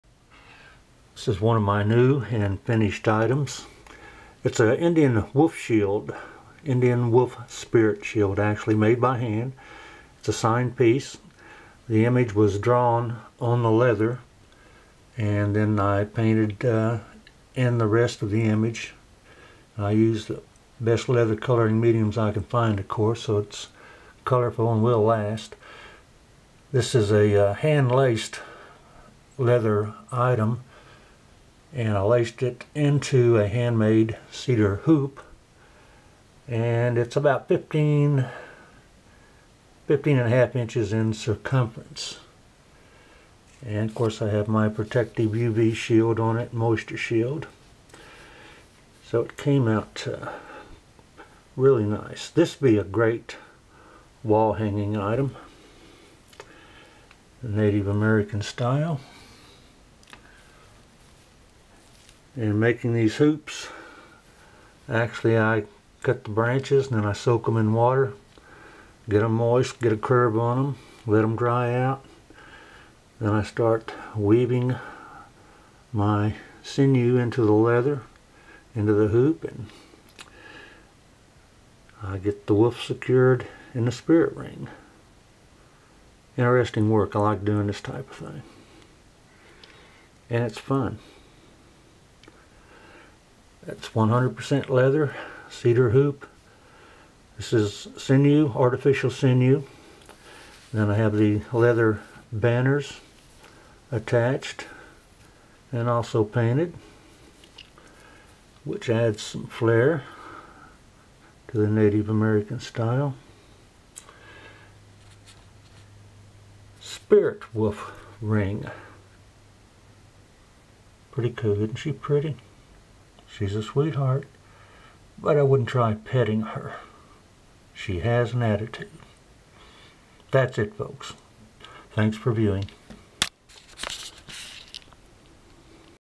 Leather Wolf Spirit Shield In Cedar Hoop
Indian Wolf Spirit Shield made by hand and is a signed piece. Image was drawn on the prepared leather and painted using the finest leather coloring mediums. Then I  hand laced the leather into a Cedar hoop which is also hand made & shaped. Item is 15 " in circumference. This  item would make a great conversation piece and would draw many compliments.

The leather is coated with a moisture and UV shield to keep it fresh and brilliant for a very
 long time. Made by hand in the U.S.A. Actually, all my work is made in the U.S.A.